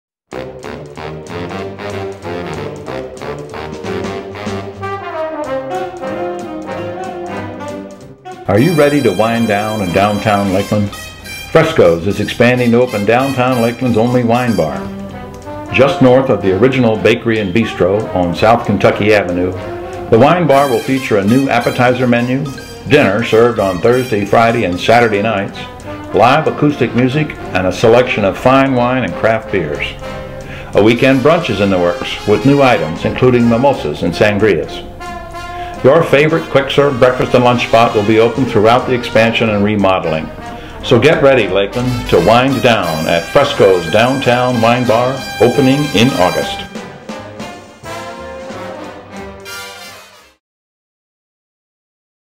Frescos DLP Spotlight Video
Beautiful downtown Lakeland's favorite quick-serve breakfast and lunch spot is getting ready for dinner and drinks. Fresco's Bakery and Bistro, on South Kentucky Avenue, is expanding into new space next door and will soon be serving up specialty appetizers, elegant dinners, fine wine and craft beers to the accompaniment of live acoustic music. Watch for the grand opening in August.